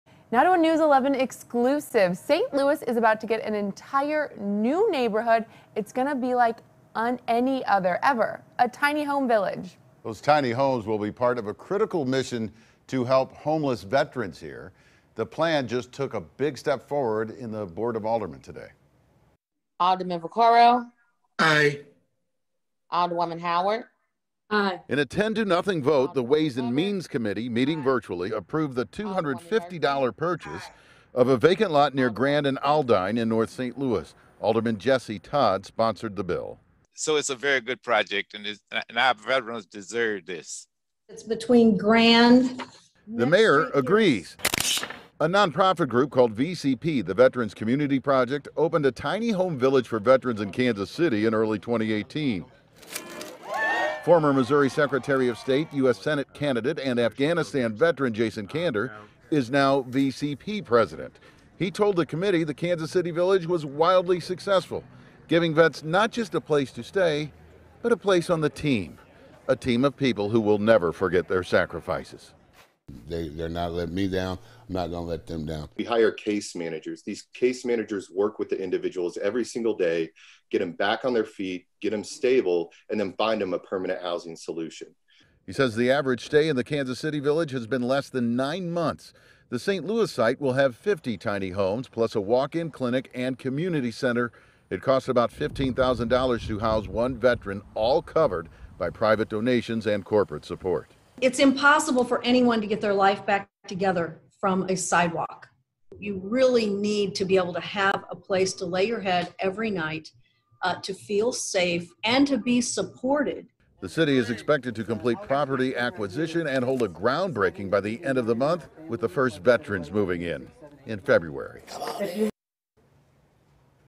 St. Louis Board of Aldermen approves land buy for village of tiny homes for veterans
St. Louis is about to get an entire new neighborhood in north city, unlike any other – a tiny home village for veterans.